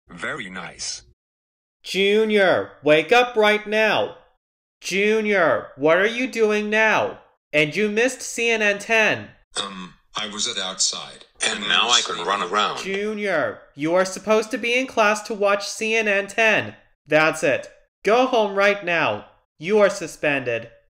Junior got in trouble for going outside and missed CNN 10
Junior misses CNN 10 and Gets Suspended